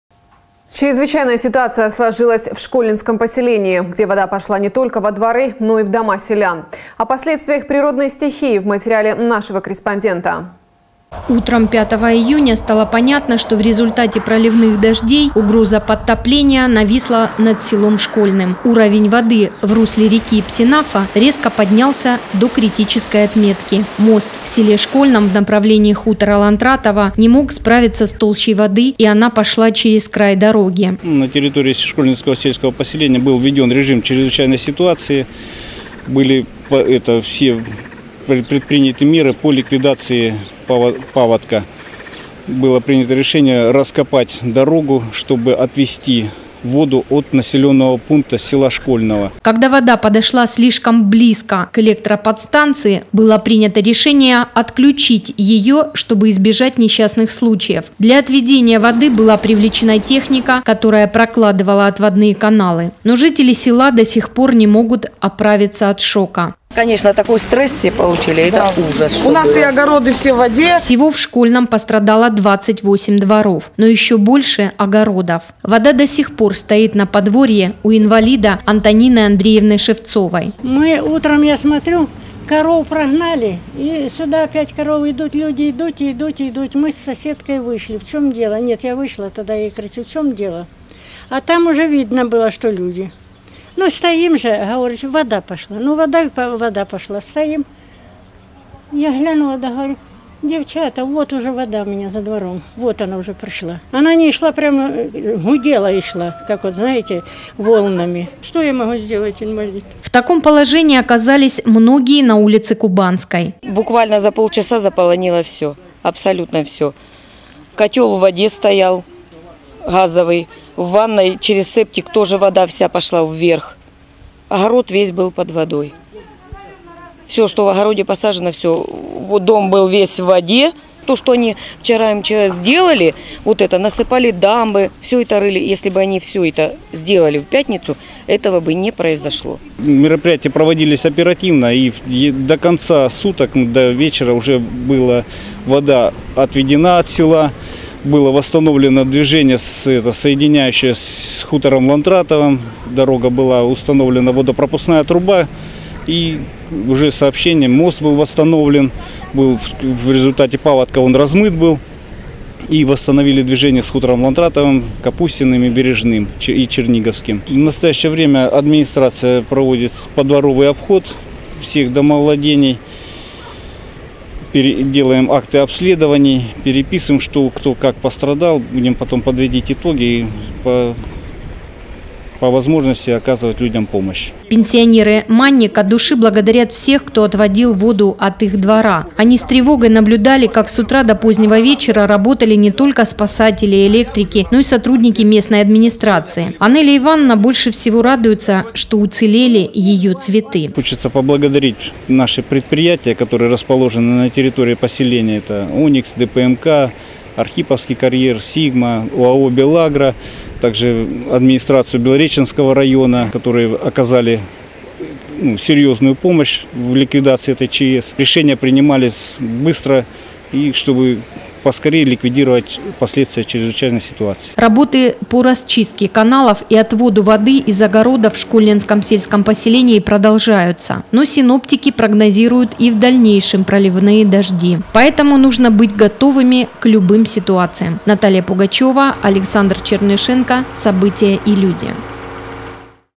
Вода сошла, но расслабляться рано
Чрезвычайная ситуация сложилась в Школьненском поселении, где вода пошла не только во дворы, но и в дома селян.